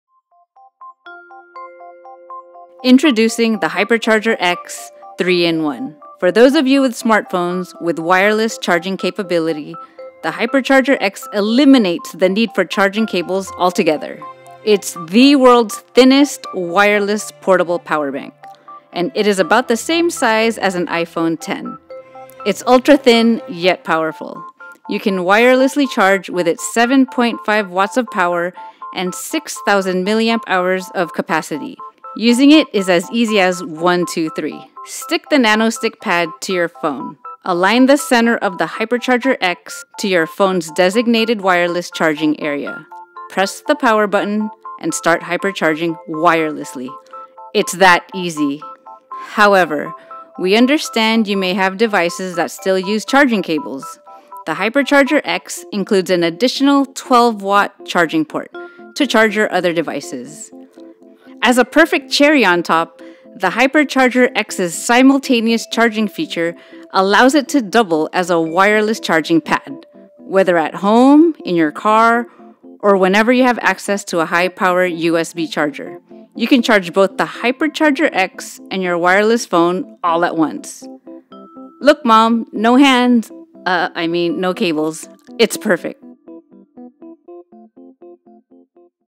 HyperCharger X - Wireless Powerbank - How it Works
HYPERCHARGER X - Wireless UltraThin HyperCharger

3-in-1:

Wireless Powerbank - Wireless Charging Pad - Normal Powerbank

Ultra-Thin Wireless Powerbank

The most powerful wireless charging powerbank for its class. 7.5 Watts of Qi-Compatible wireless charging power and 6,000mAh of capacity. It's about the same size as a iPhone X so it's ultra-thin as well as being lightweight! It is the best wireless HyperCharger in it's class.

No need to buy a separate wireless charging pad. The HyperCharger X doubles as a 7.5 Watt, High Power, Qi Compaitble Wireless Charging Pad. When internal battery is drained you can simultaneously charge the HyperCharger X and wirelessly charge your phone ALL at the same time! We recommend using a 2.4 Amp or 12 Watt USB wall charger to obtain optimal charging results.

Traditional HyperCharger Powerbank

As more smartphones are enabled with wireless charging capabilities, many of our devices still use prevailing connector standards. The HyperCharger X includes a 15 Watt universal USB charging port enabled with HyperCharging technology for both Android, Apple, and other USB devices.

Features/Accessories

Super Soft-Touch Ultra-Black finish
7.5W Qi-Compatible HyperCharging Wireless Technology
15Watt, 3.0Amp, USB HyperCharging Port
microUSB Recharging Port
Power-ON button
4 color LED Battery Capacity Meter
Hardware and Firmware Protection Circuitry
microUSB Recharging Cable
NanoStik PRO reuseable sticky pad
Instruction manual